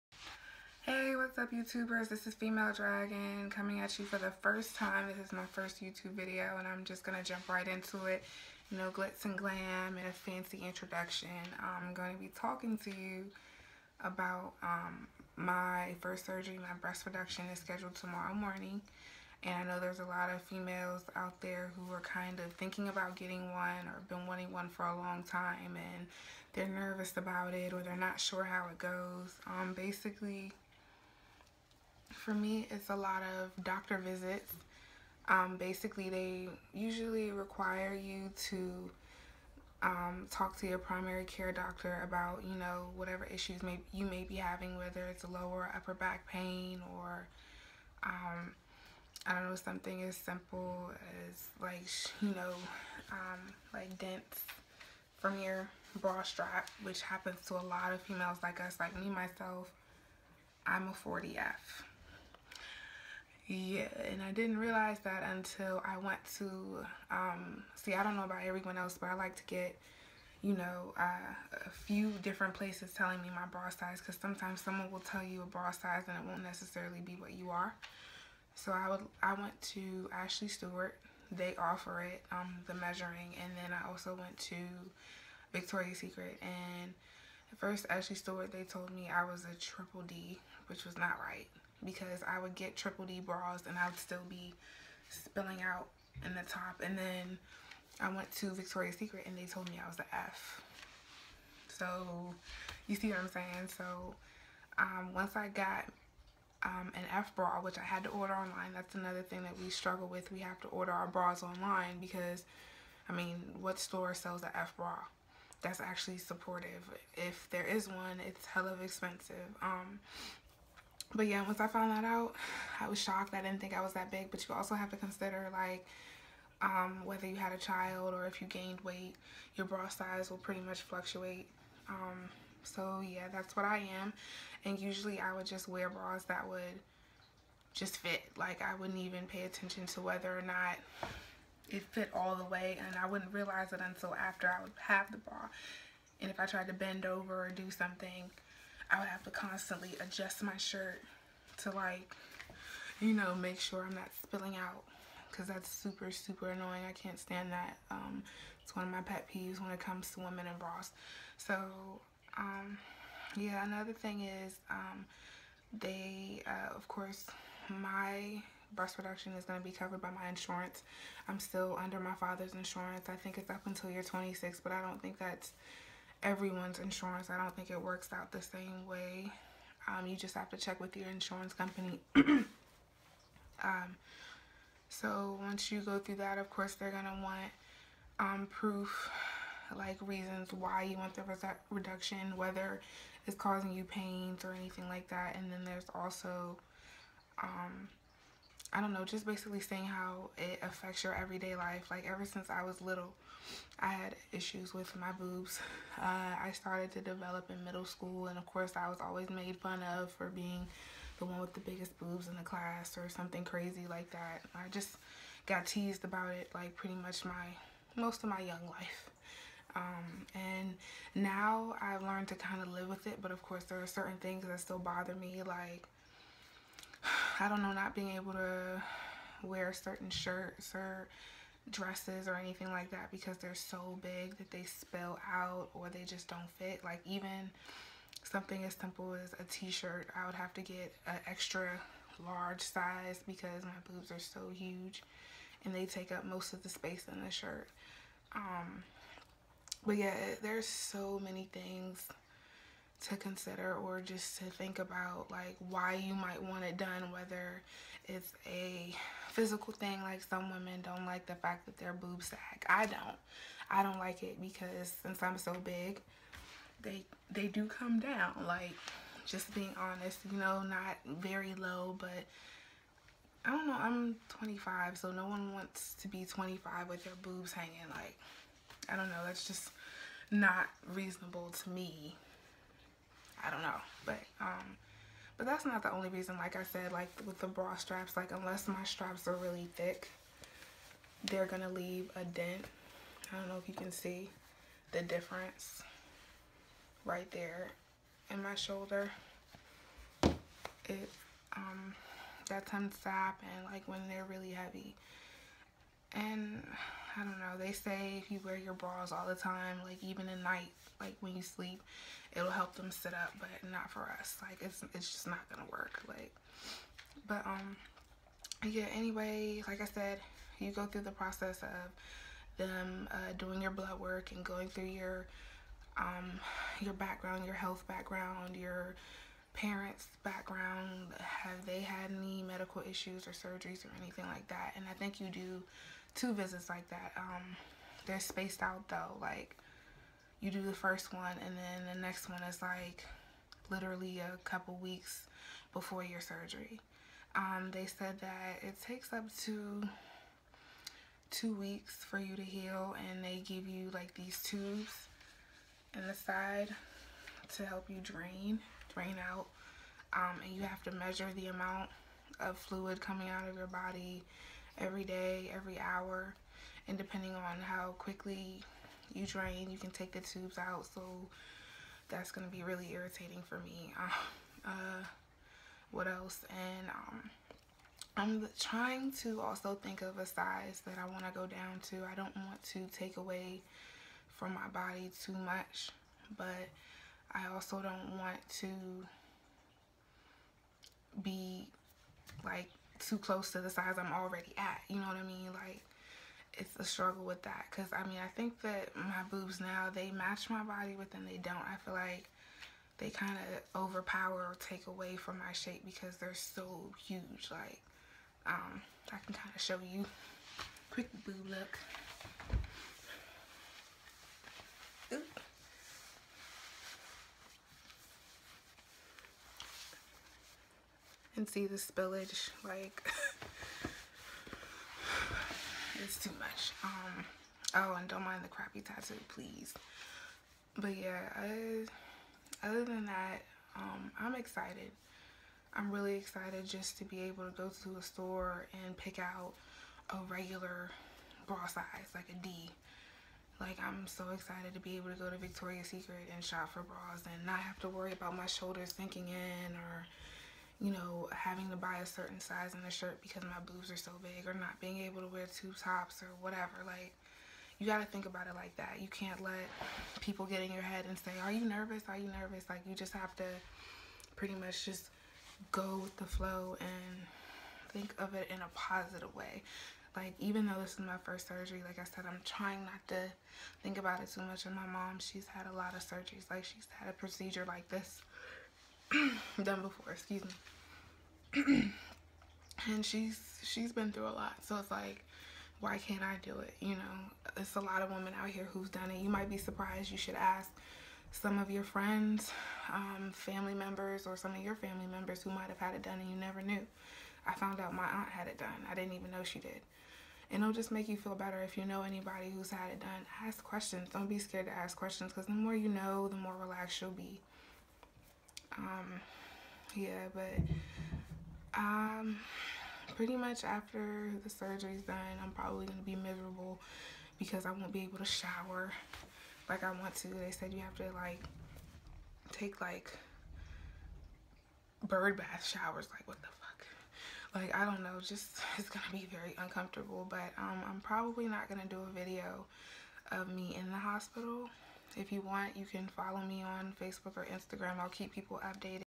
My First Surgery! Breast Reduction Talk
Sorry I look a mess but my surgery is in the morning and I didn't think it'd matter 💁🏽🙃

Anyway! Ima 40f and I plan on going down to a large D. Just basically giving you some insight on how it's gonna go and what to think about and what's needed if your going to consider. From what I've been hearing it WELL worth it. Anyone who has any experience with this kind of thing I'd love to hear it!

Fb:Robyn Kingston (ball python in my profile)